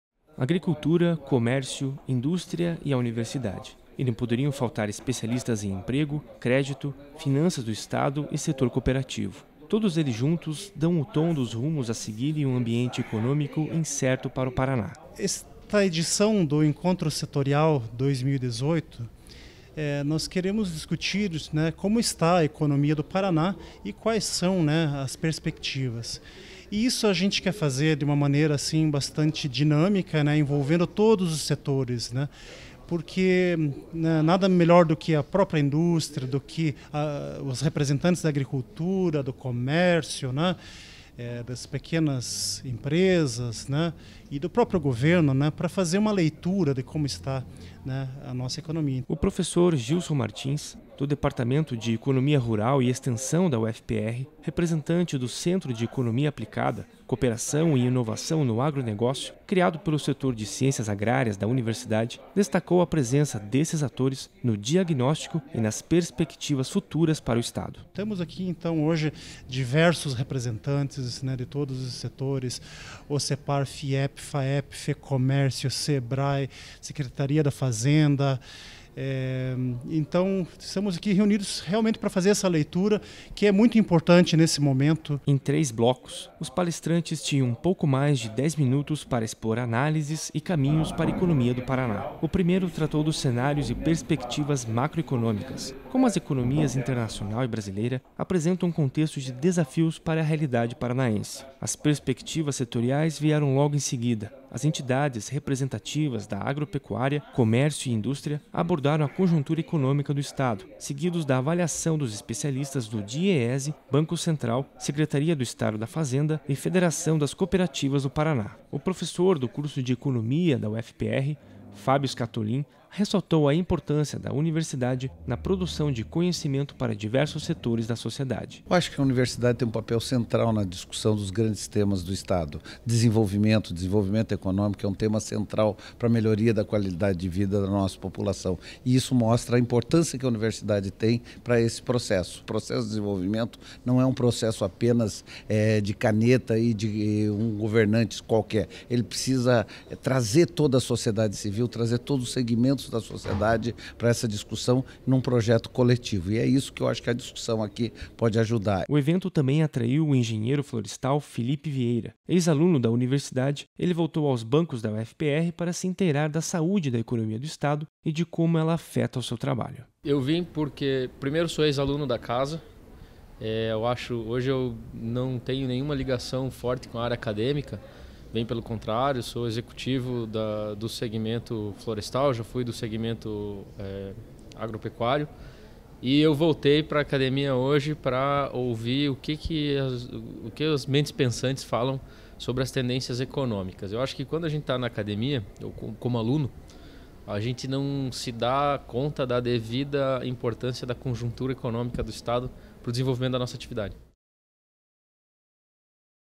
Informa | Contexto e Desafios Econômicos
A economia do Paraná foi tema de evento aqui na UFPR! O encontro setorial "Contexto e Desafios Econômicos do Paraná" debateu com vários setores da sociedade propostas para o desenvolvimento do Estado.